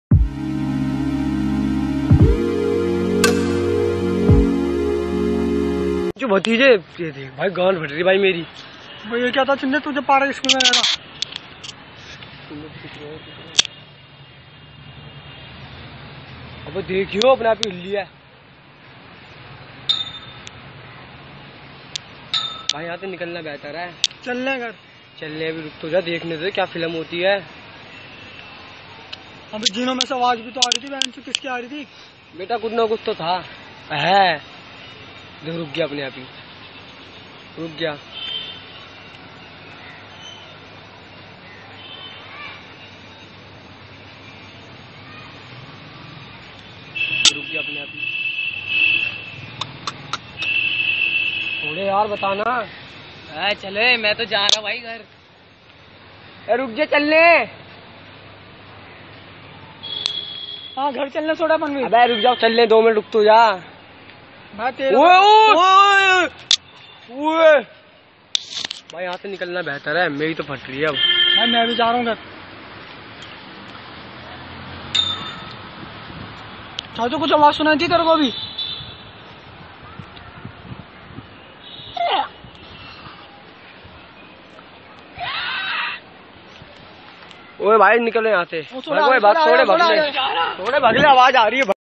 REAL GHOST | REAL GHOST SWINGING | Real ghost in swing jhoola
The video showing a real ghost swinging in a public park very scary

Entertain And Fun VIDEO"S For You YouTubers